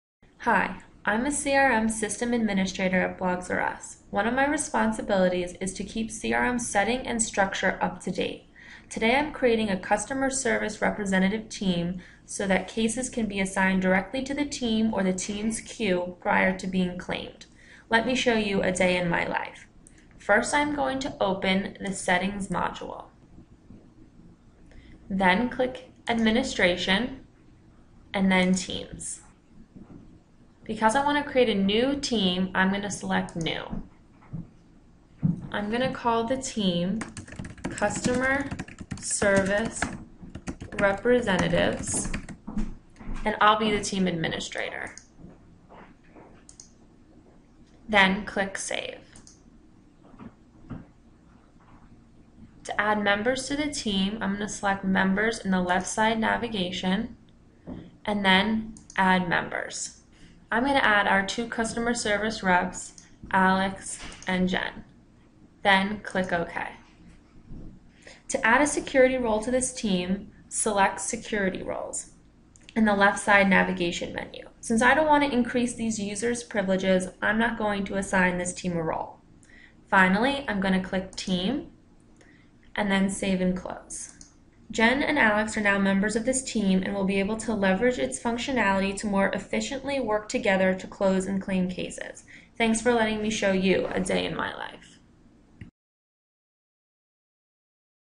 Microsoft Dynamics CRM: Creating a Team
This is the tenth installment in our new video blog -Day In The Life- a Microsoft Dynamics CRM video blog by Cole Systems. Watch as we show you how to create a team in to Microsoft Dynamics CRM.